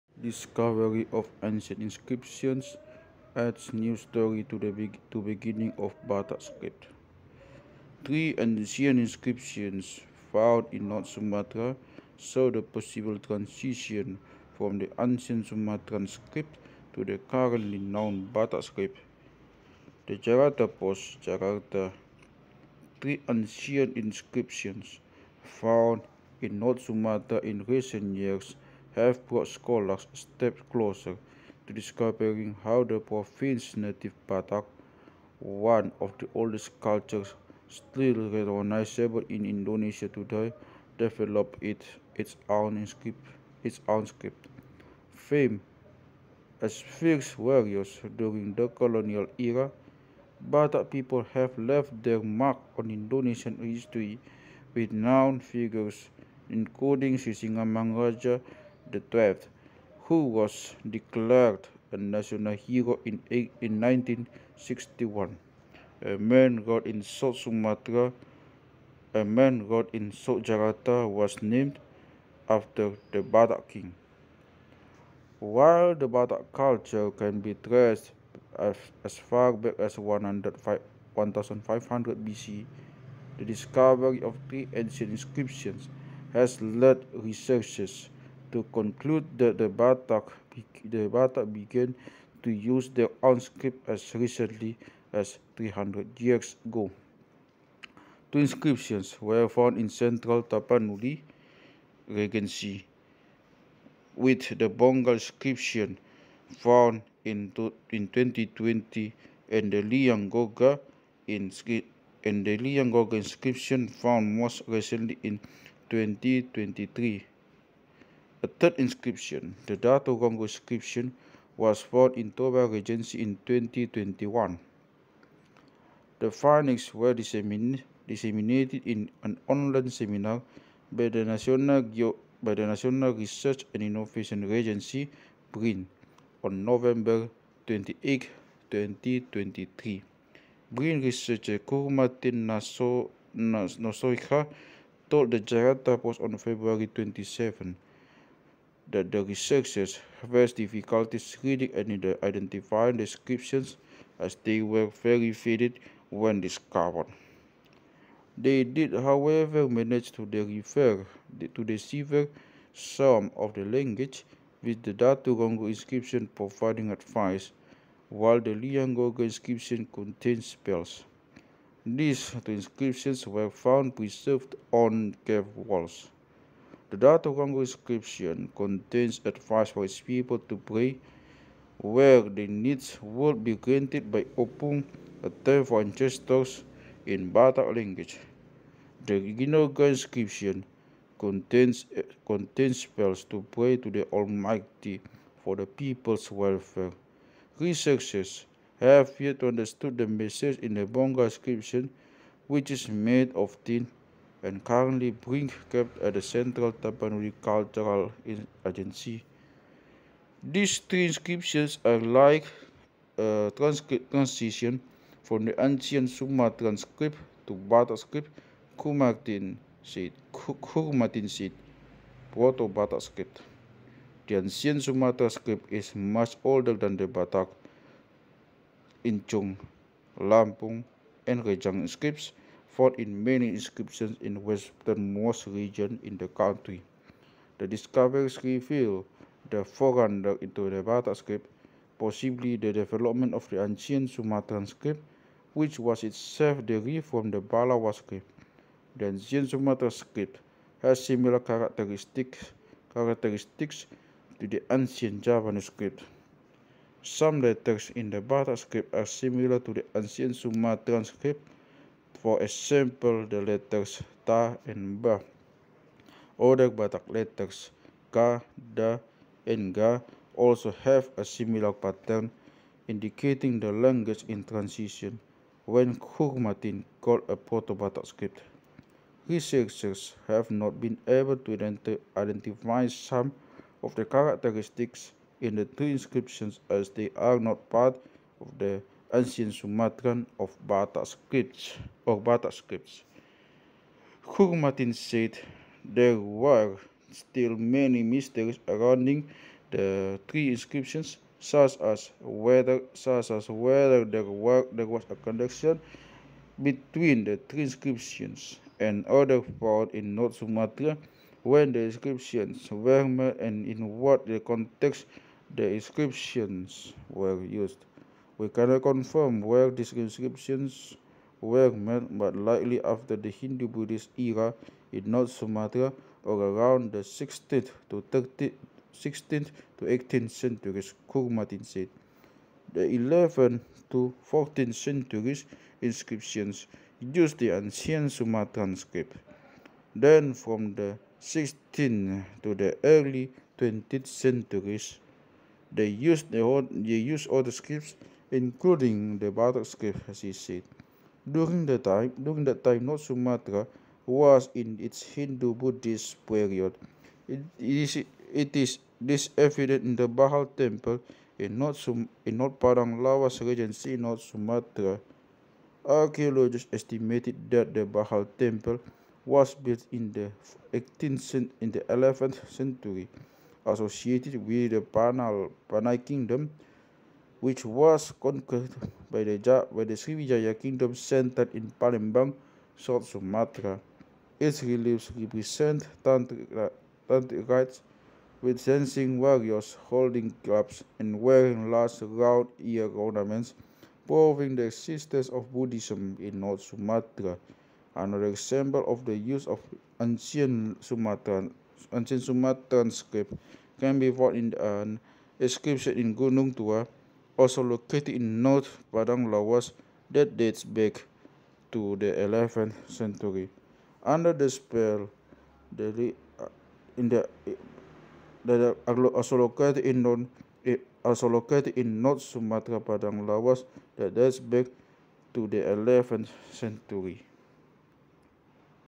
Discovery of Ancient Inscriptions Adds New Story to Beginning of Batak Script
The Jakarta Post. BRIN. 2024. Discovery of Ancient Inscriptions Adds New Story to Beginning of Batak Script. 13 Maret.